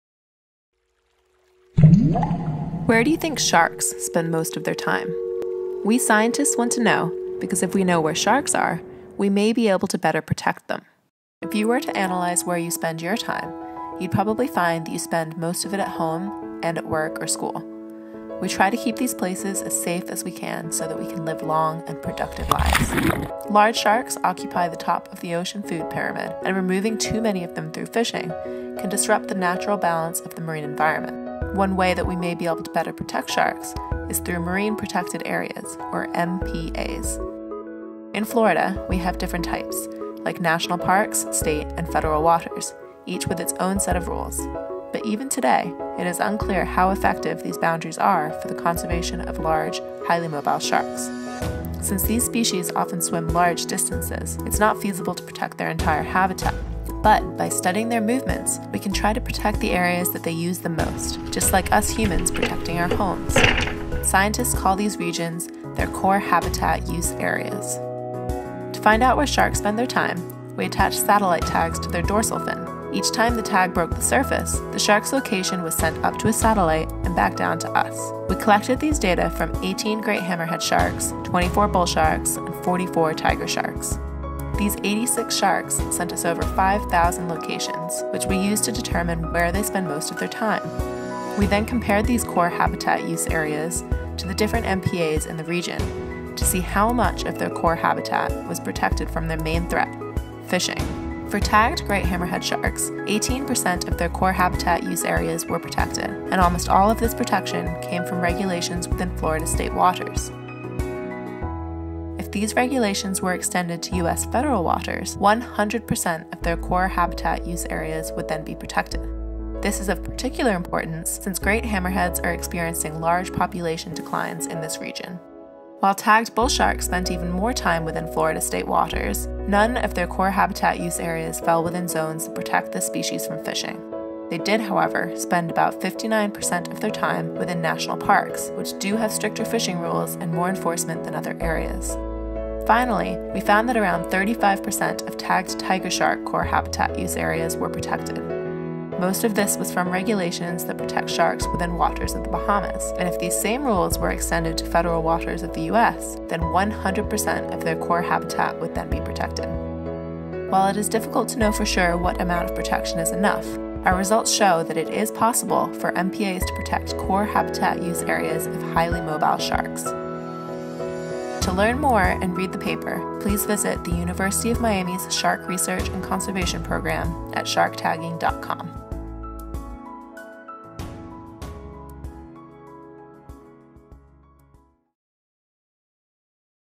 Study Says Marine Protected Areas Can Benefit Large Sharks
Researchers at the University of Miami (UM) Rosenstiel School of Marine and Atmospheric Science published new findings that suggest the expansion of protected areas into U.S. federal waters would safeguard 100 percent of core home range areas used by three species of sharks tracked in the northwestern Atlantic Ocean.